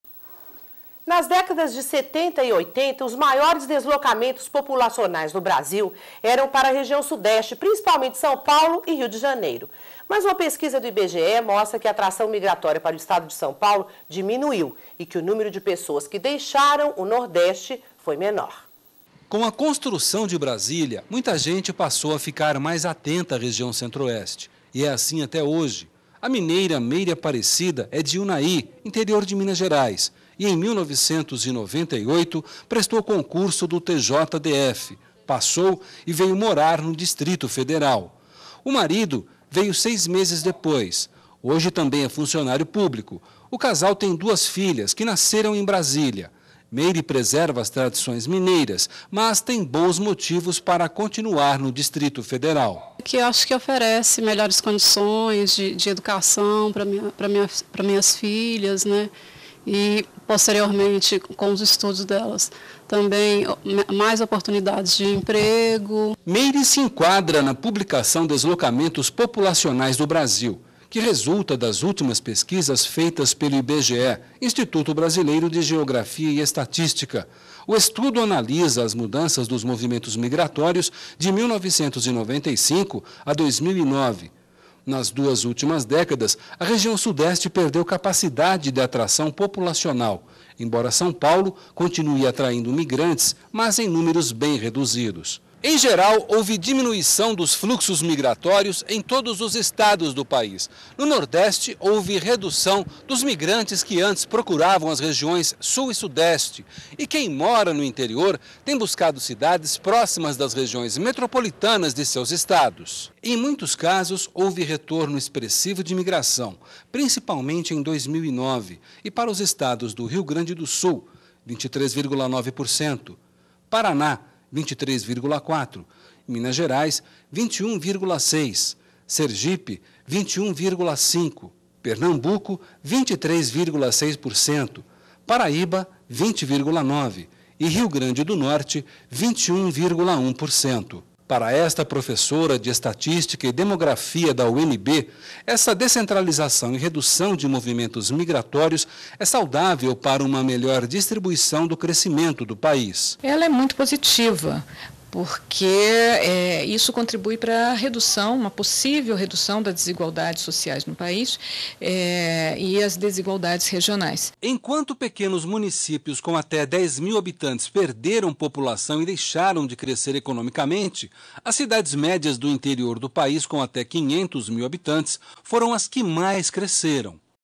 Pesquisa aponta redução na migração ao estado de SP e no nº de pessoas que deixaram o nordeste
NBR NOTÍCIAS - 18.07.11: Nas décadas de 70 e 80, os maiores deslocamentos populacionais no Brasil eram para a região sudeste, principalmente São Paulo e Rio de Janeiro. Mas uma pesquisa do IBGE mostra que a atração migratória para o estado de São Paulo diminuiu e que o número de pessoas que deixaram o nordeste foi menor.